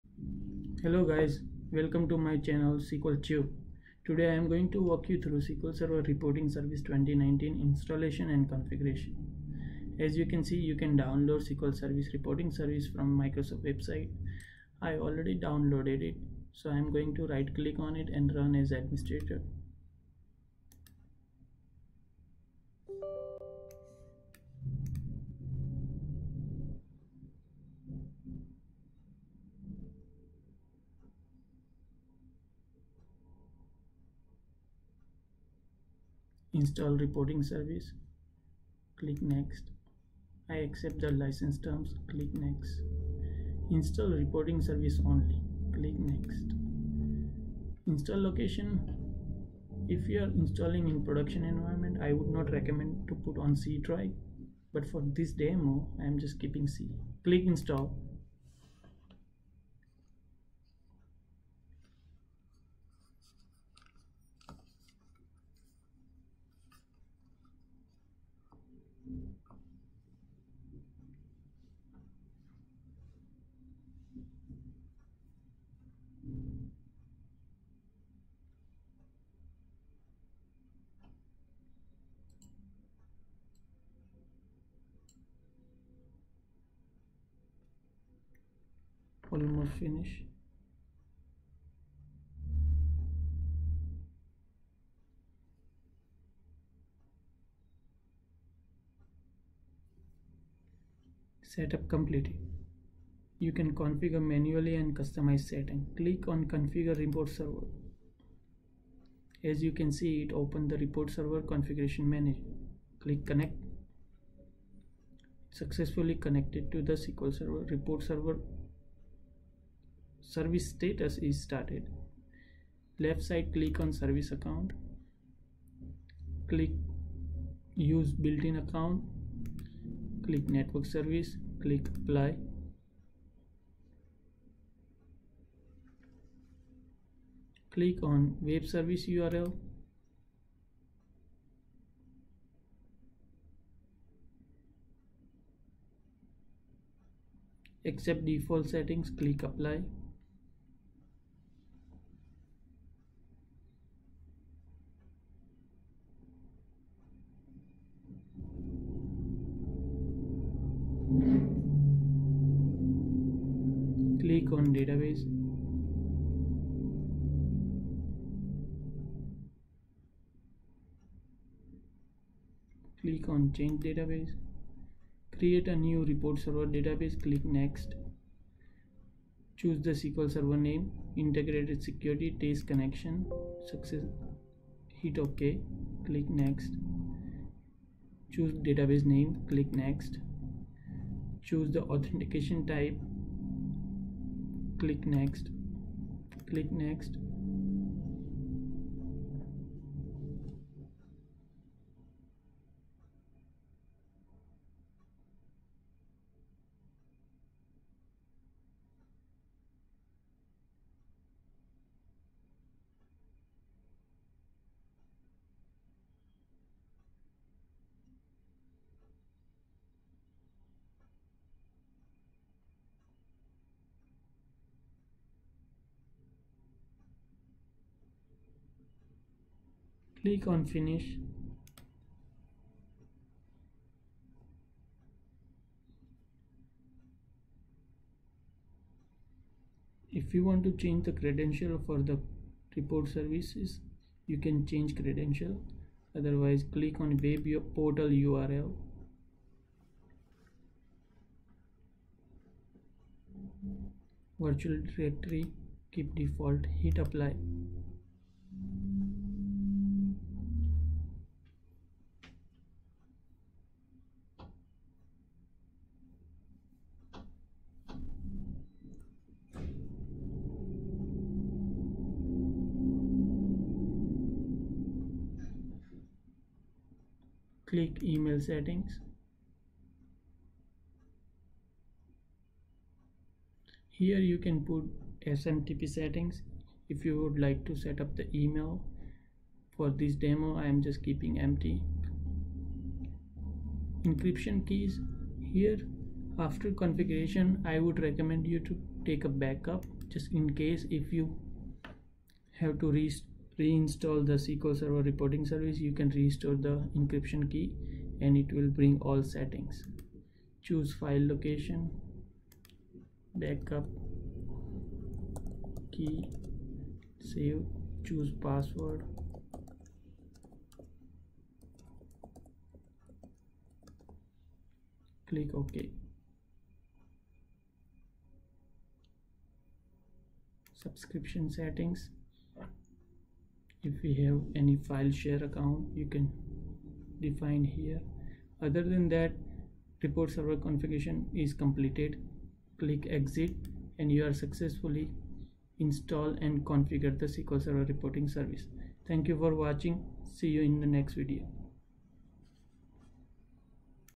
Step by Step - SQL Server Reporting Service (SSRS) 2019 Installation and Configuration for beginners
This video shows you step by step how to install and configure SSRS2019.